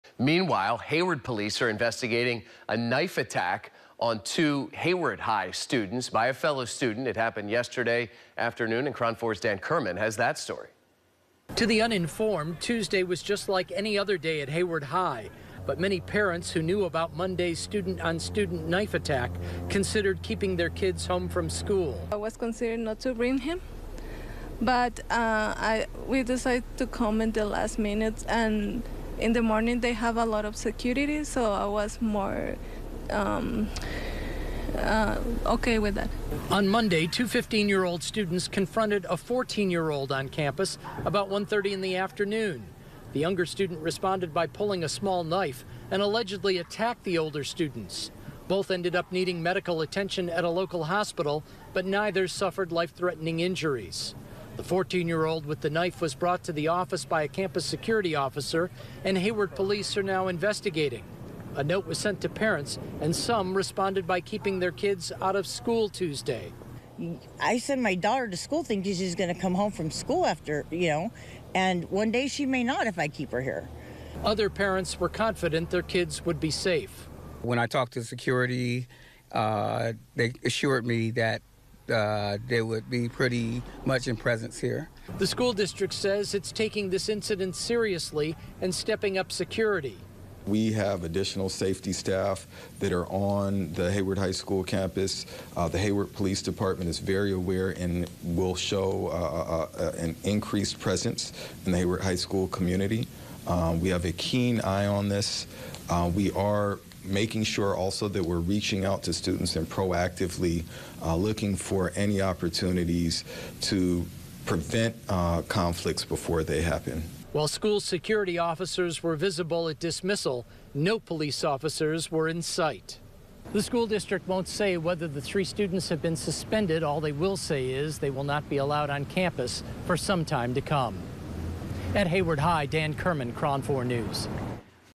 Hayward students hospitalized after knife confrontation
KRON4's Dan Kerman reports.